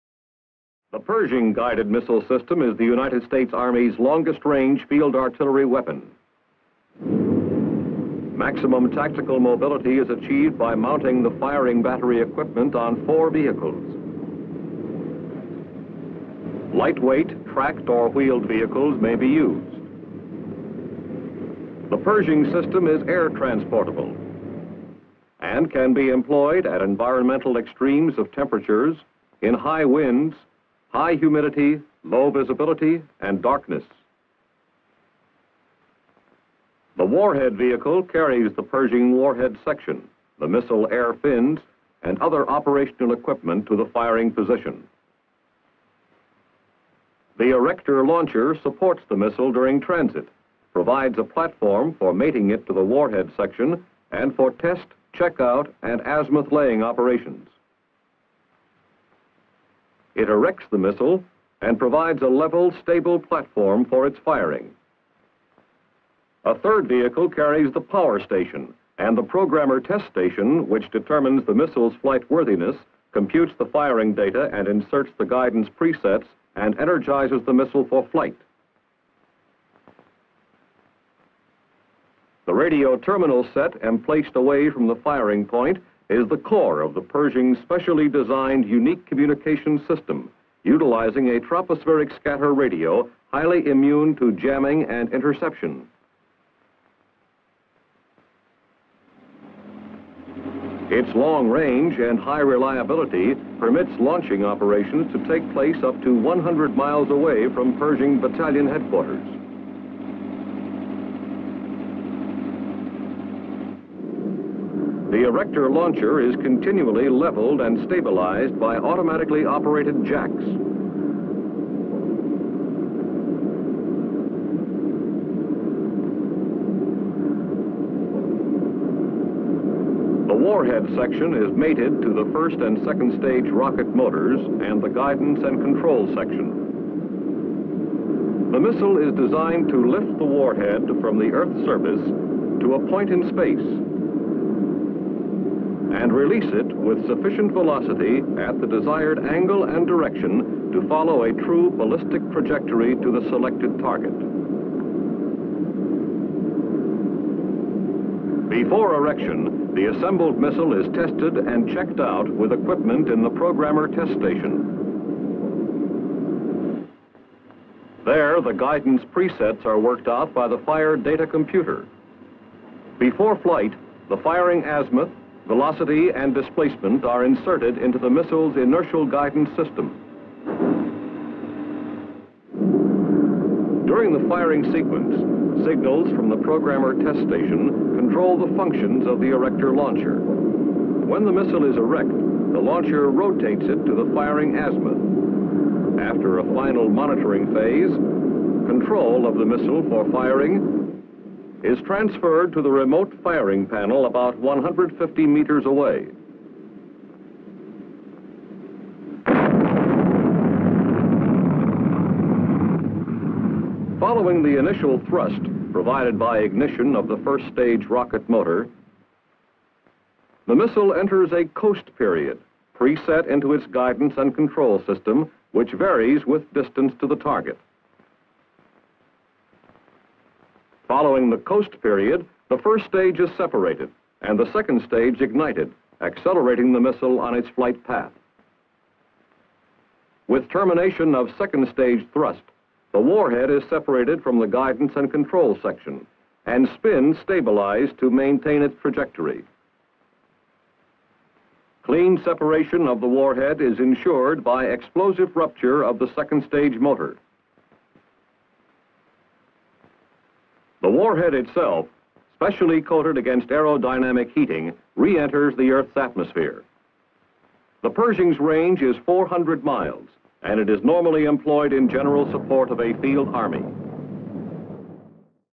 Pershing Guided Missile System - United States Army Field Artillery Weapons - CharlieDeanArchives
Features, capabilities and use of the Pershing Guided Missile System .